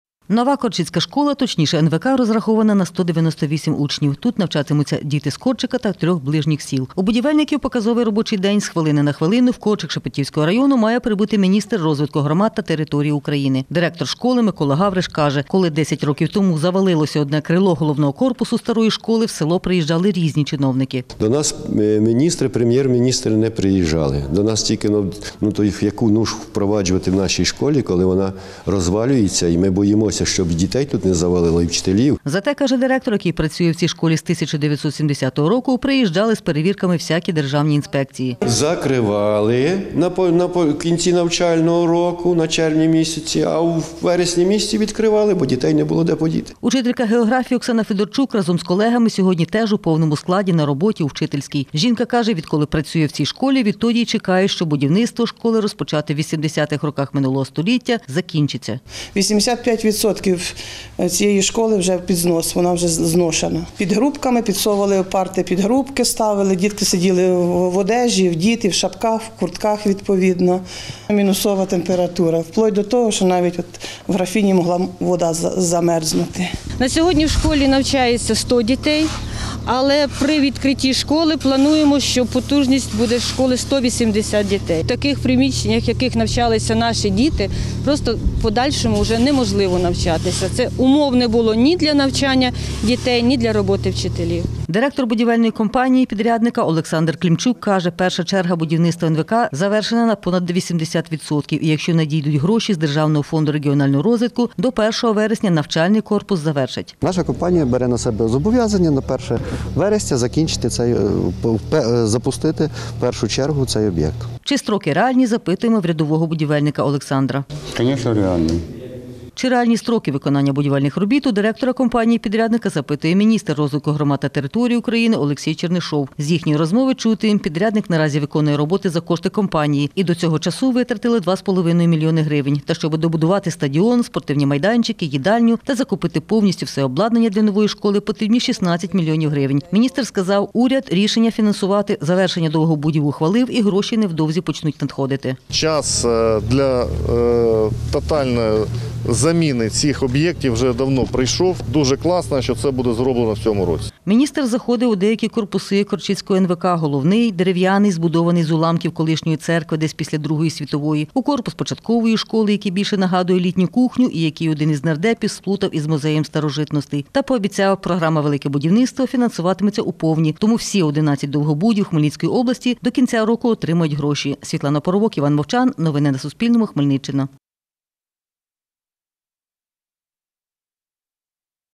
Велике будівництво: на Хмельниччині обіцяють добудувати школу, яку зводять 30 років
Корчицьку школу Шепетівського району мають здати в експлуатацію на 31-й рік будівництва. Директор будівельної компанії – підрядника «Форвард-плюс» Олександр Клімчук каже, якщо надійдуть обіцяні урядом гроші, довгобуд завершать до кінця літа, а 1 вересня школярі Корчика та трьох ближніх сіл на день знань прийдуть в нову школу.